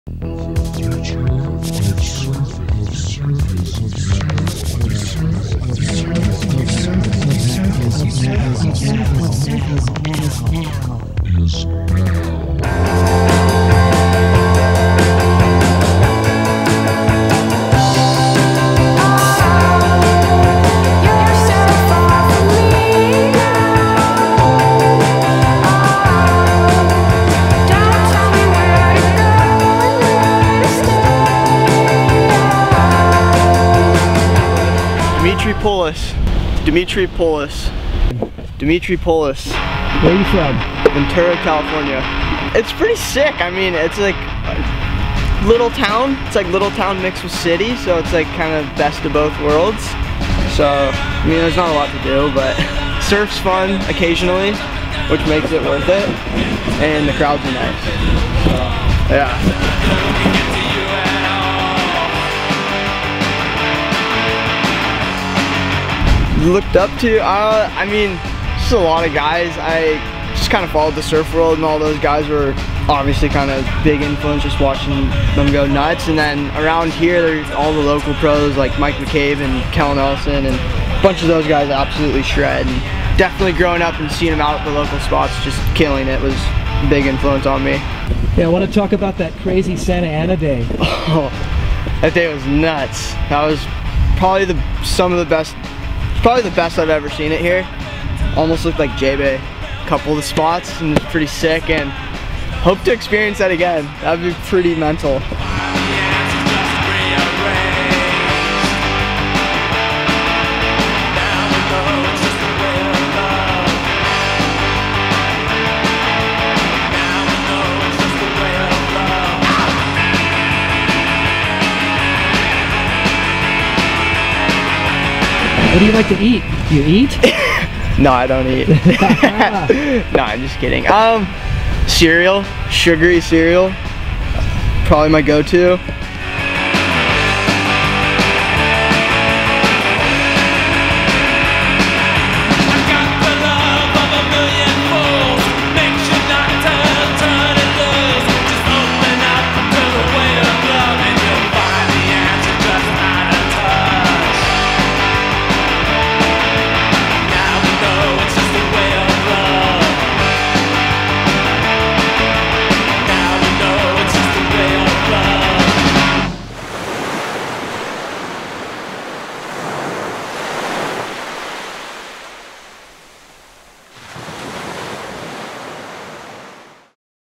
California's Rising Star: Dimitri Poulos | The O'Neill Kids Are Alright!
Whether it's his raw and refined approach to surfing or from being exposed to some of Ventura's best pro-surfers at such a young age, it's no surprise that Dimitri Poulos is quickly being recognized as one of California's best up-and-coming surfers.

Get to know Dimitri in the latest installment of "The O'Neill Kids Are Alright."